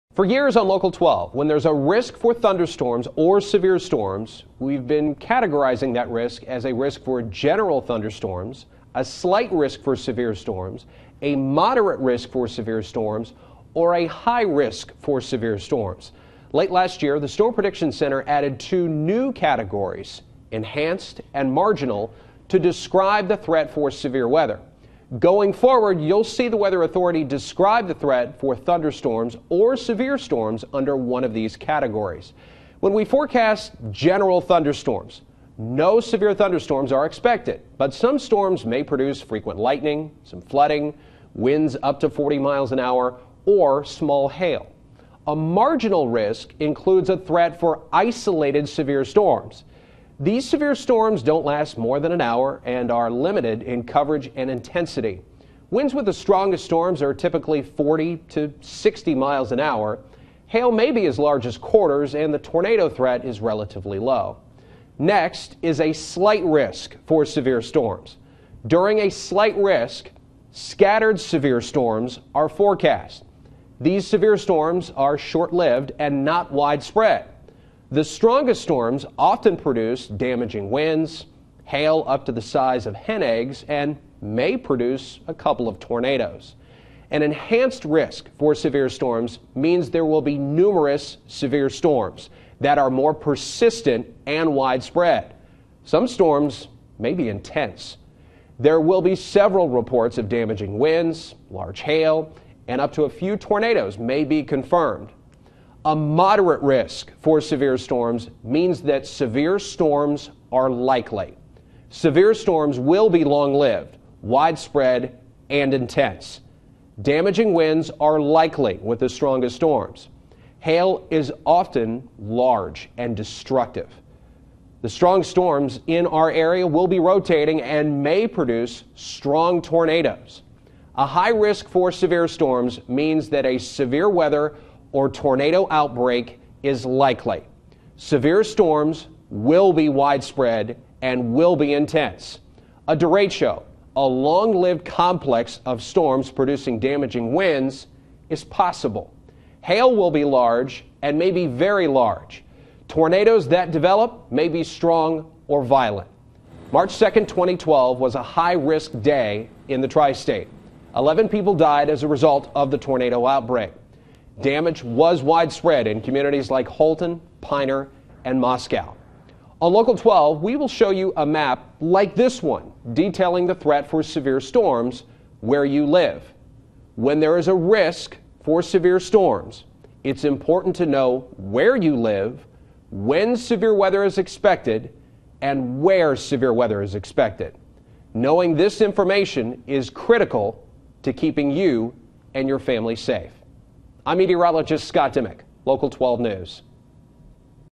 Scott Dimmich explains SPC risk categories
CINCINNATI (Scott Dimmich) -- For years on Local 12 when there's a risk for thunderstorms or severe storms, we've been categorizing that risk as a risk for general thunderstorms, a slight risk for severe storms, a moderate risk for severe storms, or a high risk for severe storms. Late last year, the Storm Prediction Center added two new categories - enhanced and marginal - to describe the threat for severe weather. Going forward, you'll see The Weather Authority describe the threat for thunderstorms or severe thunderstorms under one of these categories.When we forecast a risk for GENERAL thunderstorms, no severe storms are expected, but some storms may produce frequent lightning, some flooding, winds up to 40mph, or small hail. A MARGINAL risk includes a threat for isolated severe storms. These severe storms don't last more than an hour and are limited in coverage and intensity. Winds with the strongest storms are typically 40 to 60mph, hail may be as large as quarters, and the tornado threat is low. During a SLIGHT risk, scattered severe storms are forecast. These severe storms are short-lived and not widespread. The strongest storms often produce damaging winds, hail up to the size of hen eggs, and may produce tornadoes. An ENHANCED risk for severe storms means there will be numerous severe storms that are more persistent and widespread. Some storms may be intense. There will be several reports of damaging winds, large hail, and up to a few tornadoes may be confirmed. A MODERATE risk for severe storms means severe storms are likely. Severe storms will be long-lived, widespread, and intense. Damaging winds are likely with the strongest storms. Hail is often large and destructive. The strong storms will be rotating and may produce strong tornadoes. A HIGH risk for severe storms means that a severe weather or tornado outbreak is likely. Severe storms will be widespread and intense. A derecho - a long-lived complex of storms producing damaging winds - is possible. Hail will be large and may be very large. Tornadoes that develop may be strong or violent. March 2, 2012 was a high risk day in the Tri-State. 11 people died from tornadoes as a result of the outbreak. Damage was widespread in communities like Holton, Piner, and Moscow. On Local 12, we will show you a map detailing the threat for severe storms where you live. When there is a risk for severe storms, it's important to know where you live, when severe weather is expected, and where severe weather is expected. Knowing this important is critical to keeping you and your family safe.